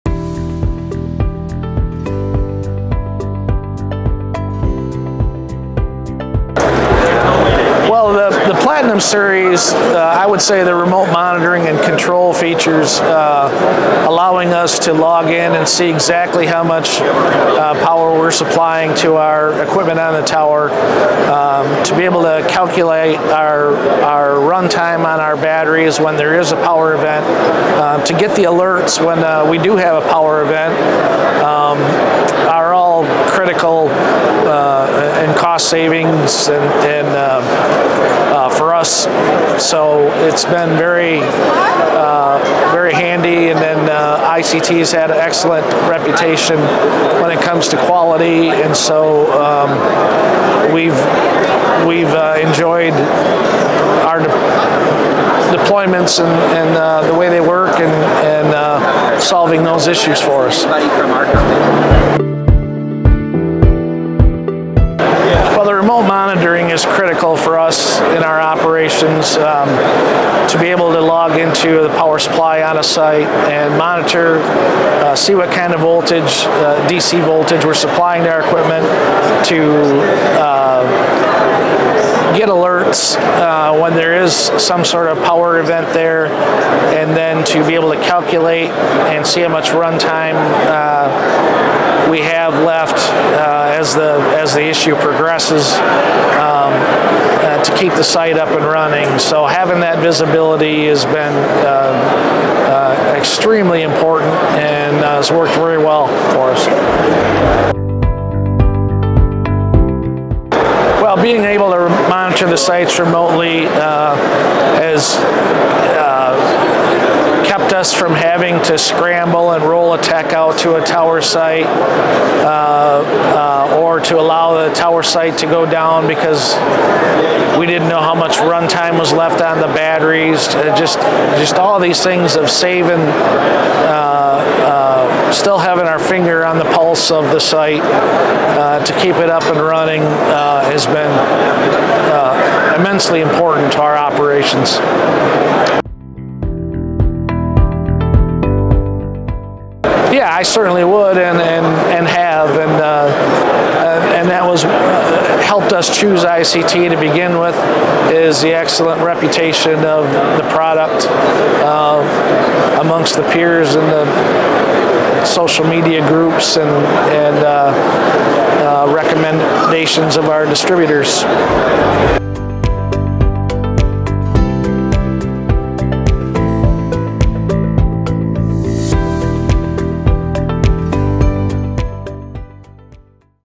ICT Customer Insights - ISP Management
In this Customer Insights episode, Jeff Hall, President of ISP Management in Michigan, shares why ICT’s Platinum Series is his favorite DC power solution. With remote monitoring and power control, ISP Management has significantly reduced technician visits, leading to cost savings and improved operational efficiency.

Jeff emphasizes that remote monitoring is critical for their operations and that ICT’s system works exceptionally well, giving them full visibility and control over their network’s power infrastructure.

He also highlights ICT’s strong reputation for quality, making them a trusted partner in maintaining a stable and resilient network.

Watch this brief overview to see how ICT’s solutions help ISPs like ISP Management cut costs, enhance network performance, and improve remote management.